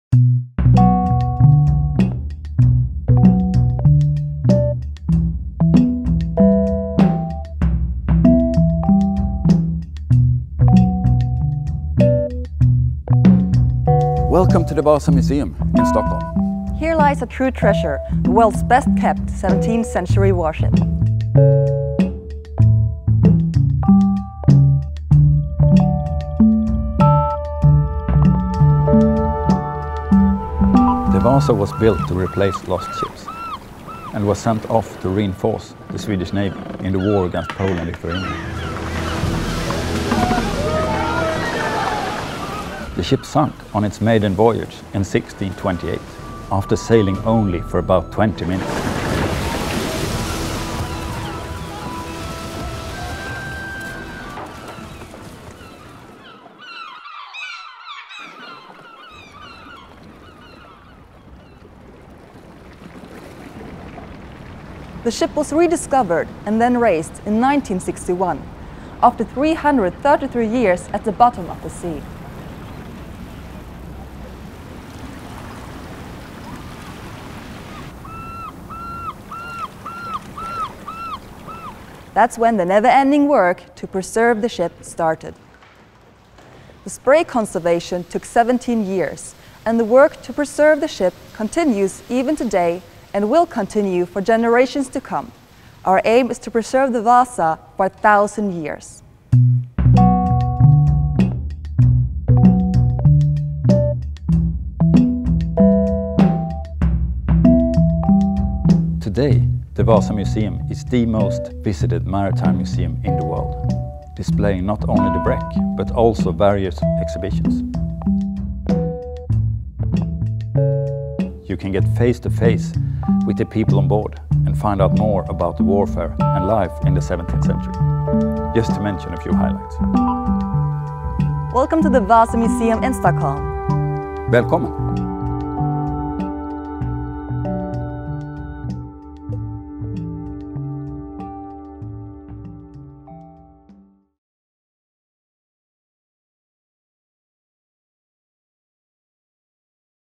This is the Vasa Museum
Welcome to the Vasa Museum in Stockholm, the home of the worlds best kept 17th century warship.